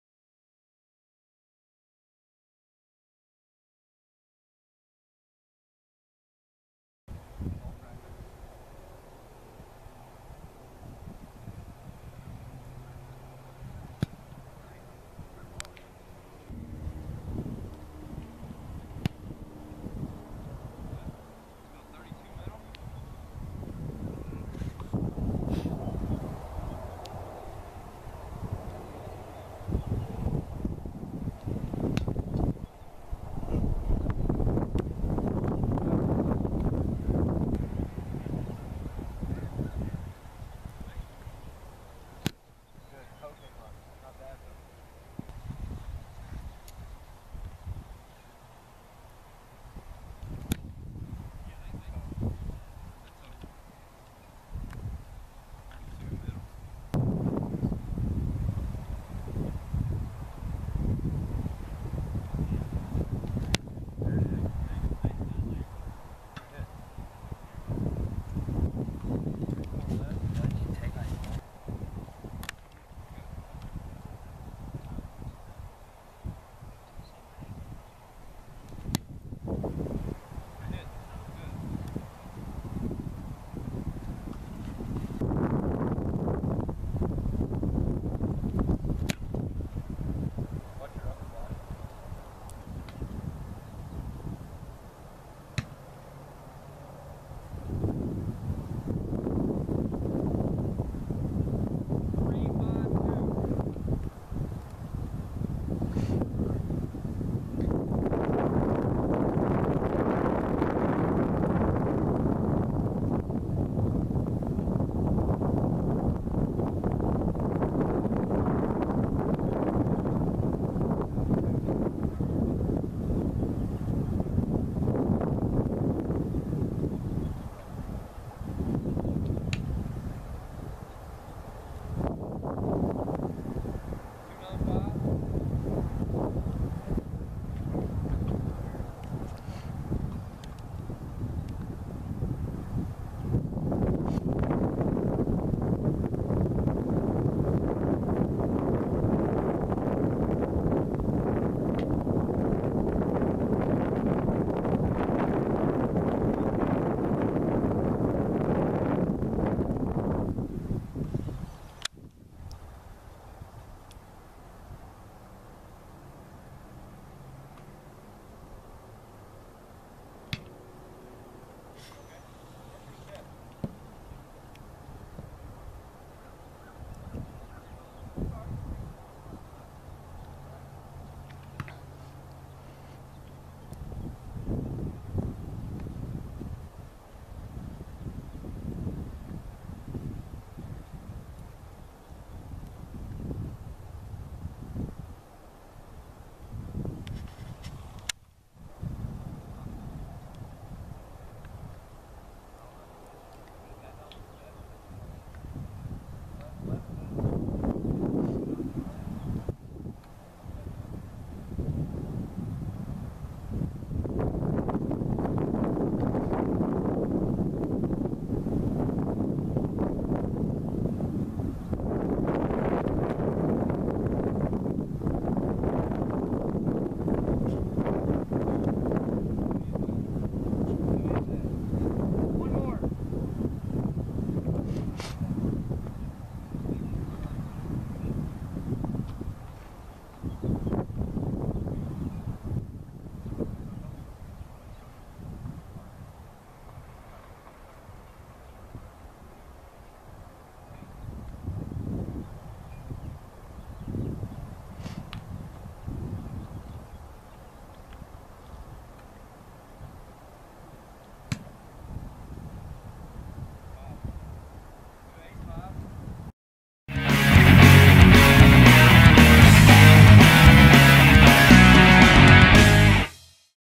Will Patterson - Elite Kicking Clinic Jan 25th, 2014
Will is charting field goals and punting at the Oxford, MS Kicking clinic hosted by Elite Kicking.